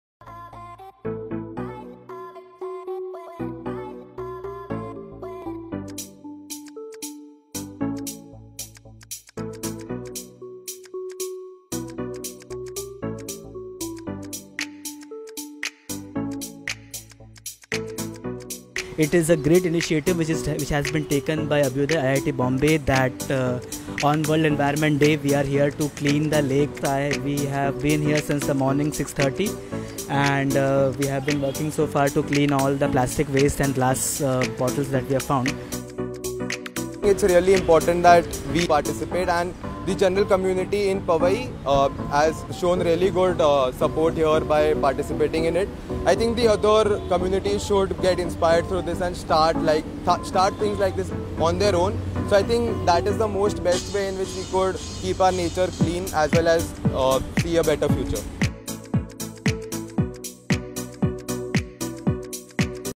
Powai Lake cleanup
18th June 2019:

“A clean environment is a human right like any other. It is, therefore, part of our responsibility towards others to ensure that the world we pass on is as healthy, if not healthier than we found it .” – Dalai Lama

On the occasion of World Environment Day, 5th June 2019, Abhyuday, IIT Bombay conducted the Powai Lake Cleanup Campaign in collaboration with Nisarg Swasthya Sansthan which was followed by the plantation drive on the banks of the Lake. More than 400 volunteers actively participated to become a part of the change and build a better, safer and cleaner society.

We hope to see such more initiatives soon!